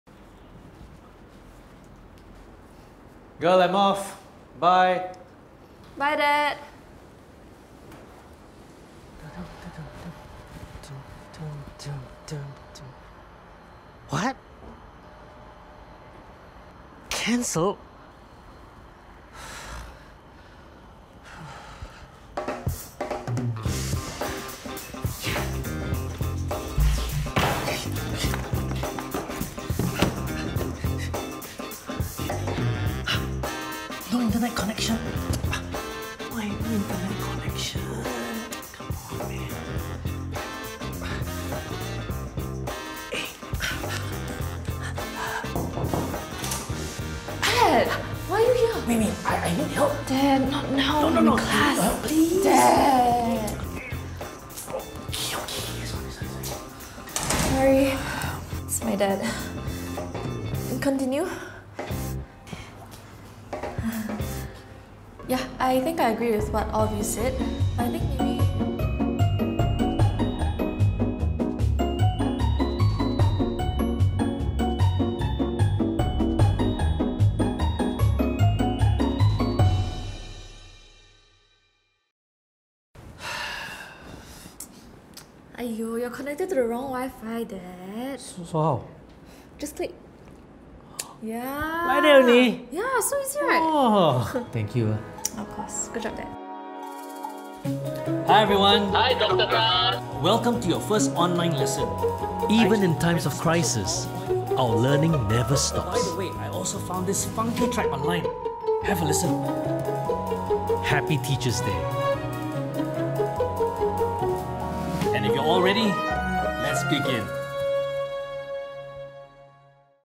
A Tribute to ...
During this time of crisis, it is even more important for us to continue learning. This is our light-hearted tribute to that someone who makes a great impact on our lives. Watch till the end to find out who that is!